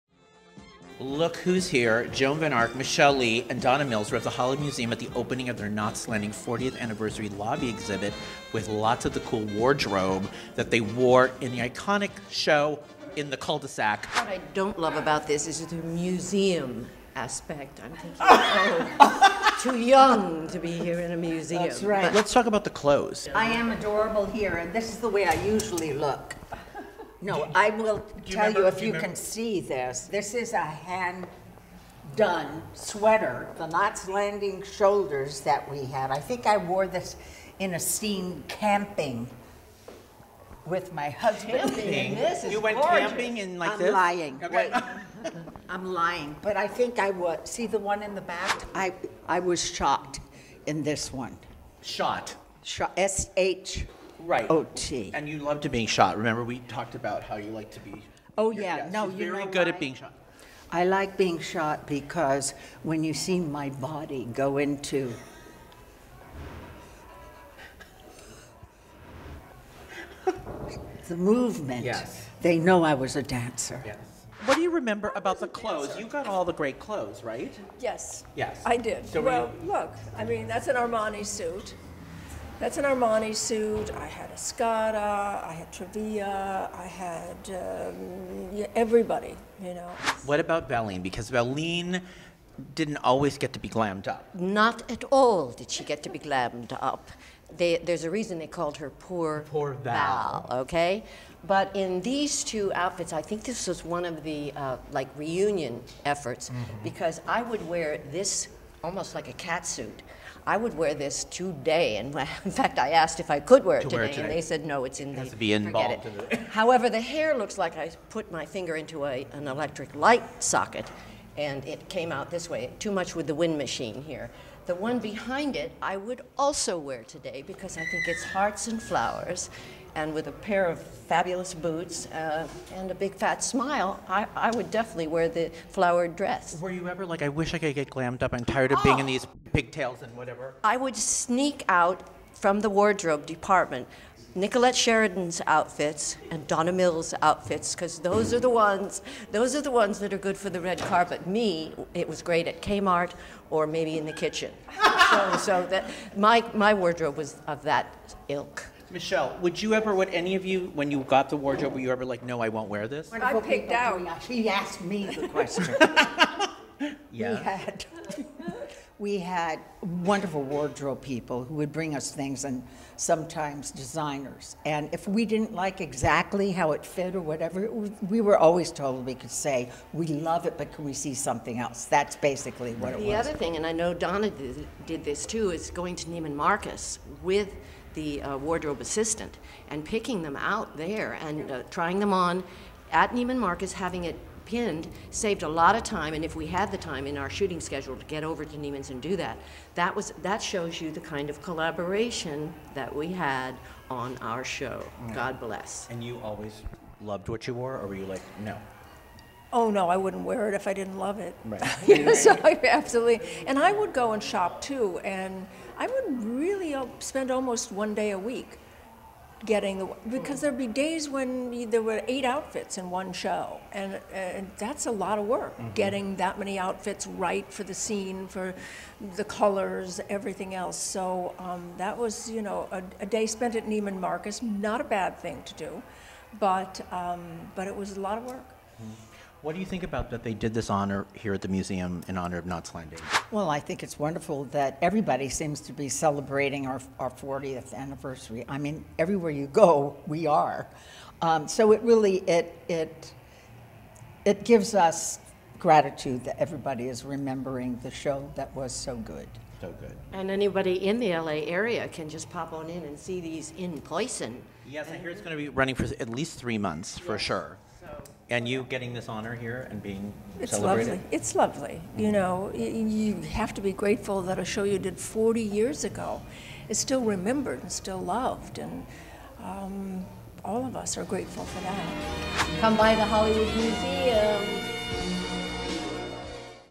Joan Van Ark, Michele Lee & Donna Mills Hollywood Museum Interview - Knots Landing 40th Anniversary
In celebration of Knots Landing's 40th anniversary, the Hollywood Museum unveiled its first lobby exhibit of 2020 featuring the costumes worn by the three leading ladies of the series: Joan Van Ark (Val), Michele Lee (Karen) and Donna Mills (Abby).

Michael Fairman was on the scene with the Michael Fairman Channel for a behind-the-scenes sneak peek before it was shown to the public on opening night.  Michael chatted with Joan, Michele, and Donna to get their thoughts on the wardrobe their characters wore during the series run from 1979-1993, the exhibit itself, and the show being loved and remembered by so many people at this special time in its history.